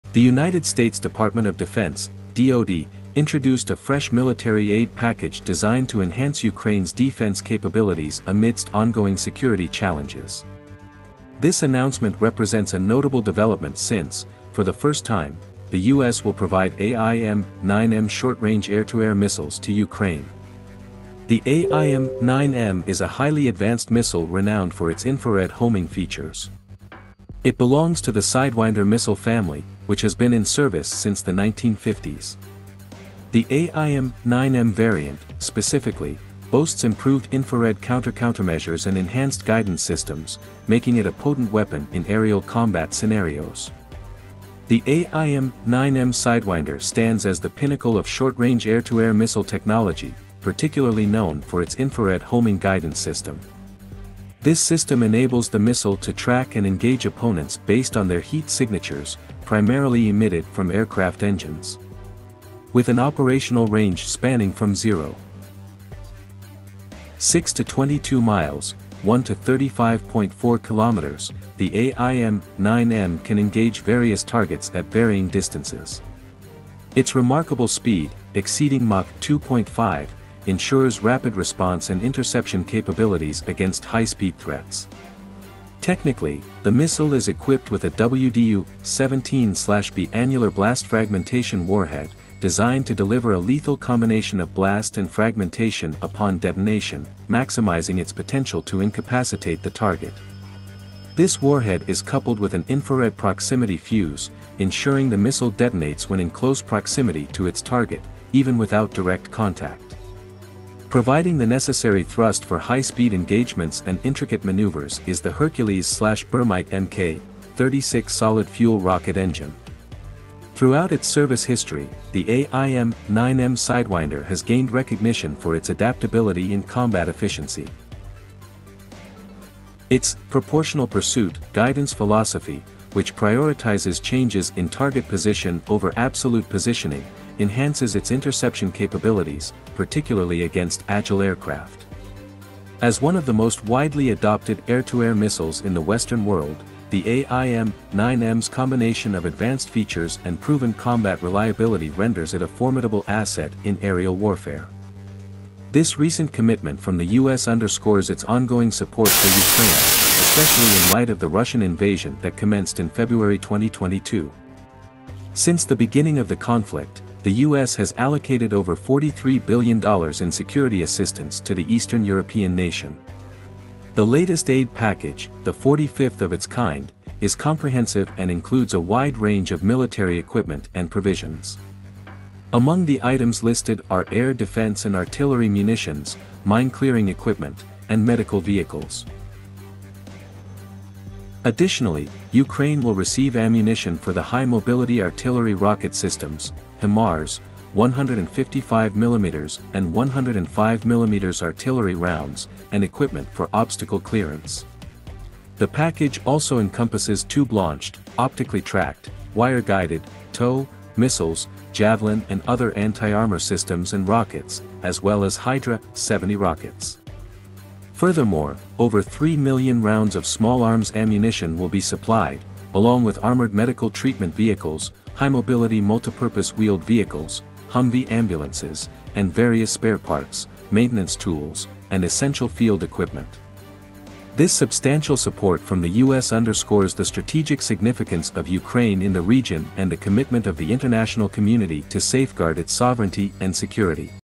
US Boosts Ukraine's Defense with First Delivery of AIM 9M Air to Air Missiles
On August 29, 2023, the United States Department of Defense (DoD) unveiled a new military aid package aimed at bolstering Ukraine's defense capabilities amidst ongoing security challenges. This announcement marks a significant development as, for the first time, the U.S. will be delivering AIM-9M short-range air-to-air missiles to Ukraine.